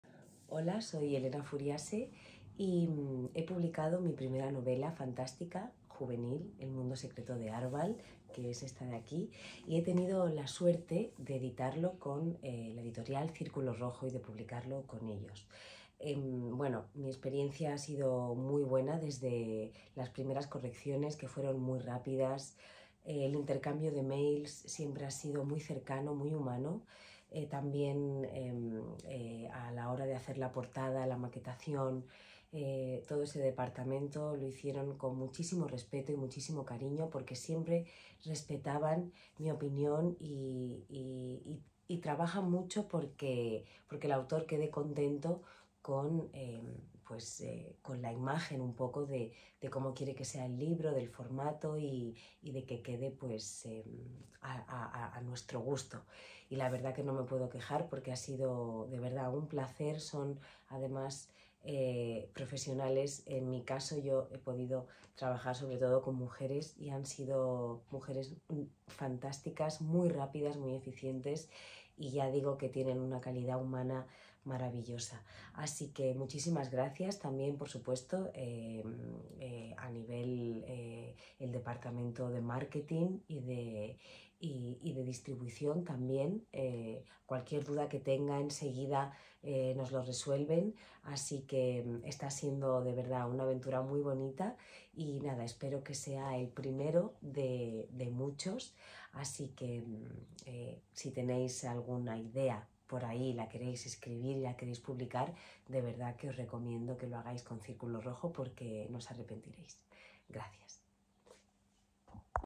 Elena Furiase cuenta su experiencia con Editorial Círculo Rojo
La actriz acaba de publicar su primer libro y ha confiado en Editorial Círculo Rojo. Aquí os dejamos sus opiniones sobre todo el proceso de edición y su experiencia a la hora de editar un libro.